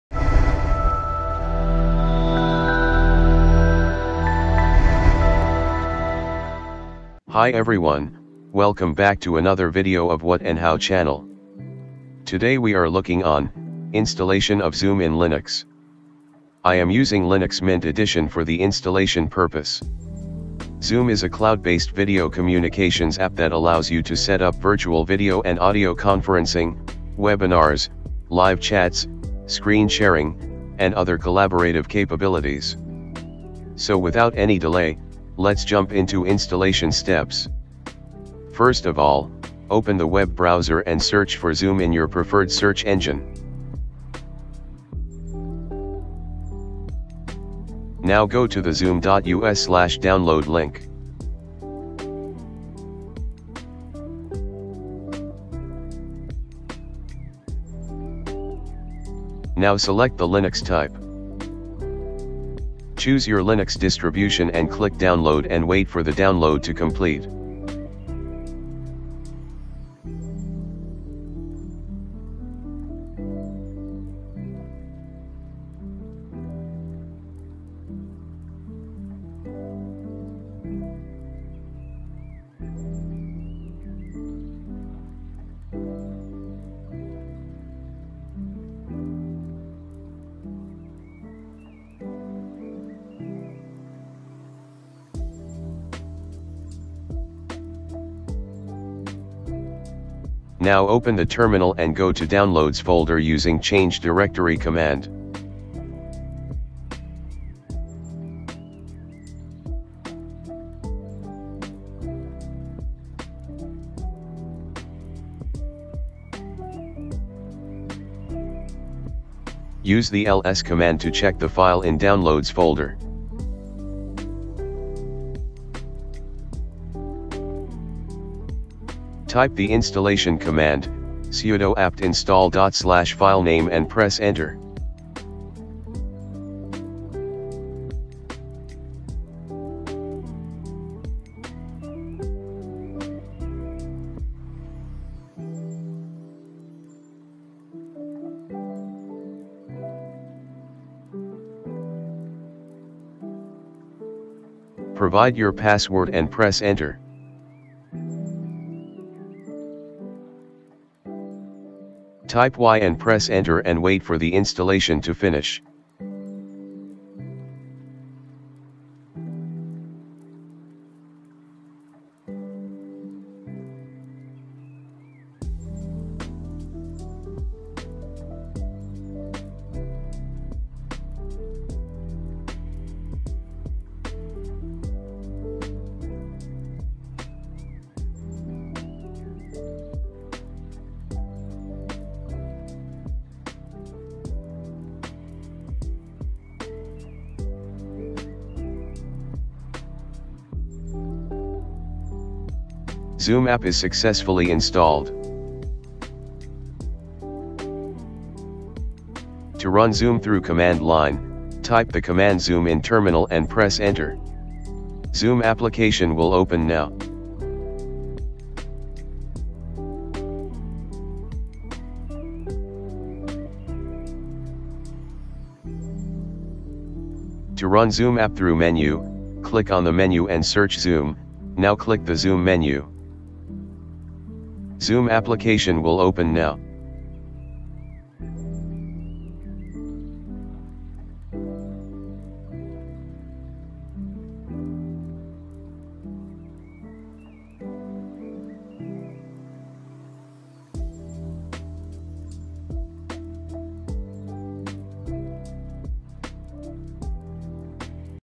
How to install Zoom on Linux Mint, Ubuntu, Other Linux Distributions
A simple installation guide on how to install the latest version of zoom in linux debian distributions (Ubuntu, linux mint etc).